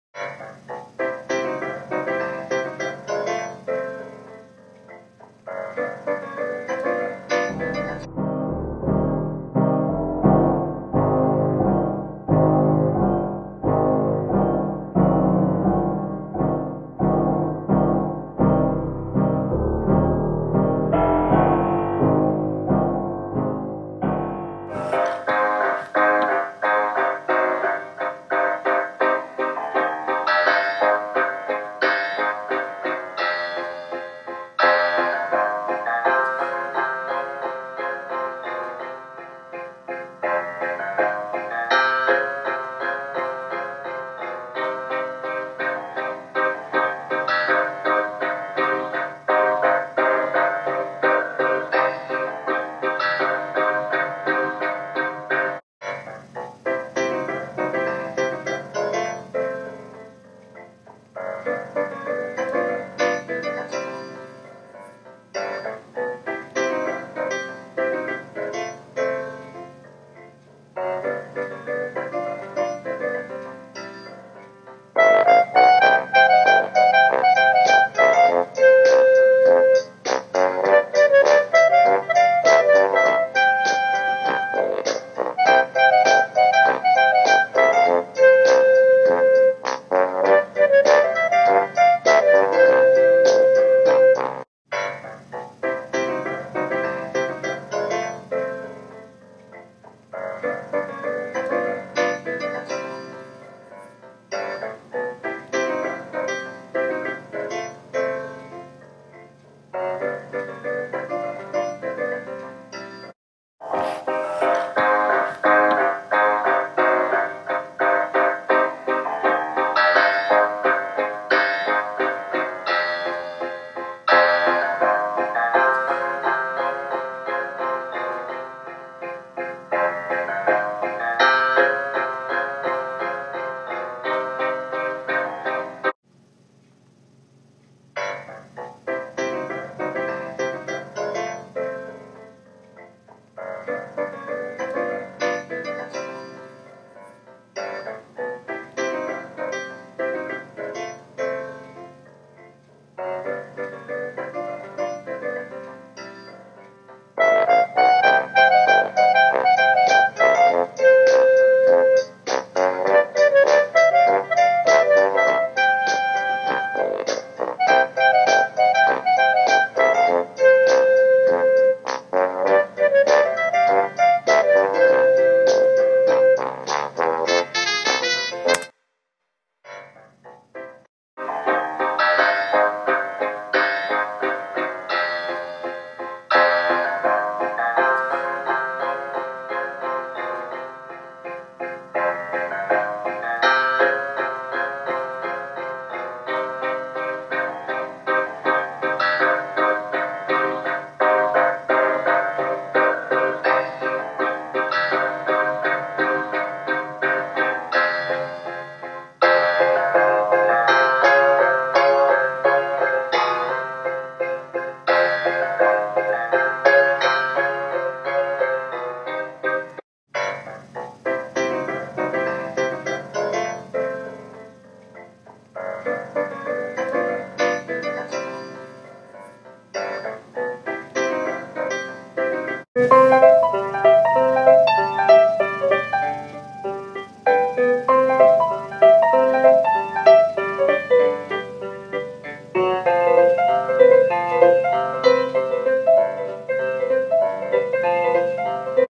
Silent Film#2
Bailee Dougherty, Lydia Sigler, and Courtney Wheeler, used the iOS app 'Vintagio' to make this creative piece for 8th grade Technology Trends class.